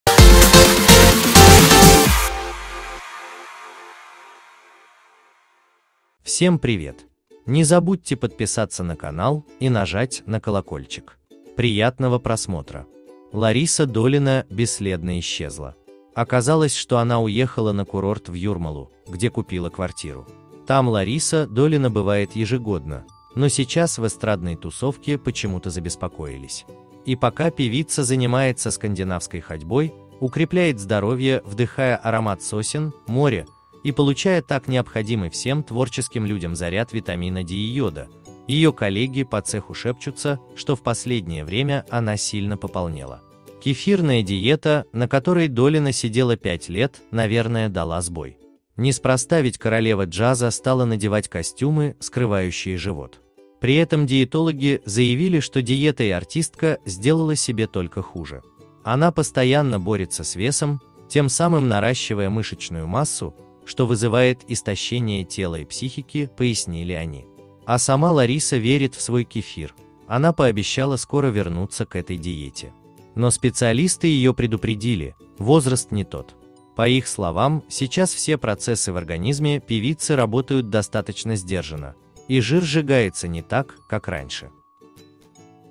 Лариса Долина бесследно исчезла
Оказалось, что она уехала на курорт в Юрмалу, где купила квартиру. Там Лариса Долина бывает ежегодно, но сейчас в эстрадной тусовке почему-то забеспокоились.